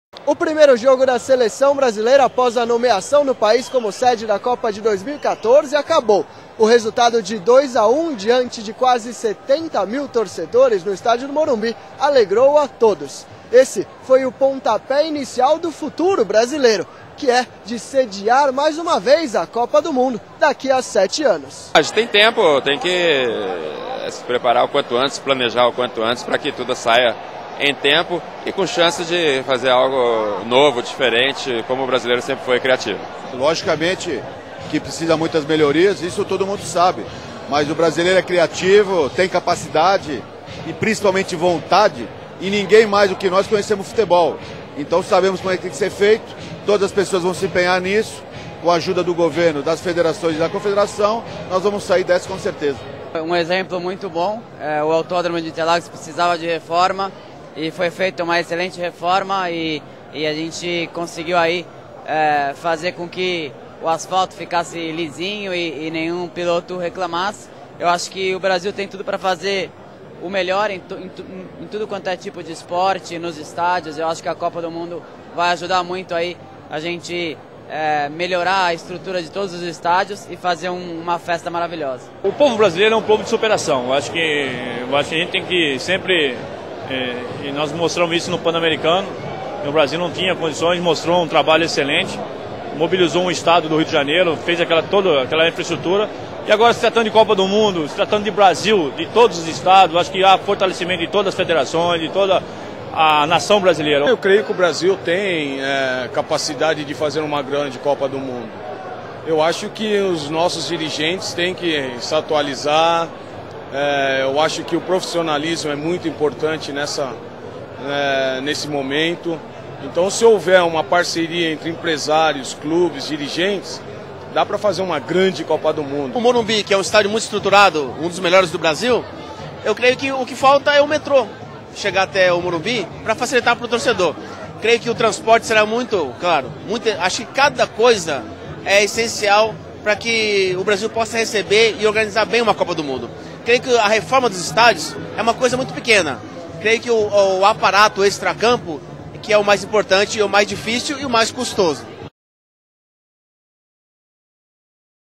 Camarote VIP no Jogo Brasil Vs. Uruguai no Morumbi
vídeo das entrevistas feitas no camarote vip da cbf e fpf no jogo brasil e uruguai que aconteceu no morumbi em são paulo em 2007.

[TV FPF]
Repórter: Guilherme Buso;
Cinegrafista & Editor: Adriano Bacalá.

entrevistados:
raí
andrés sanches
felipe massa
lê
paulo sérigo do nascimento
müller

21-nov-2007